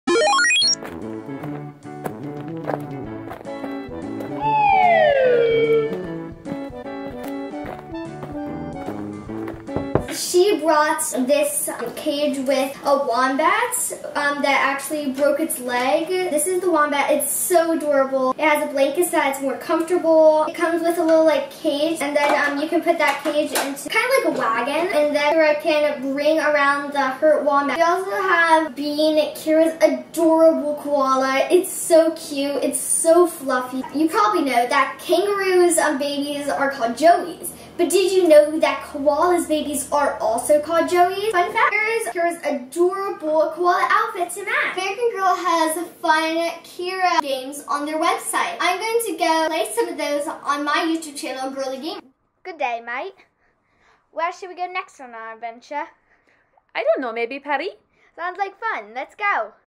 Kira Bailey Doll Giveaway Trailer Video! Last Day to Enter is TODAY!
Today is the last day to enter to win a Kira Bailey American Girl doll! To enter, head over to watch the full video on The Sammie and Georgie Show Then, click the link in the description of the video to get credit for your entry!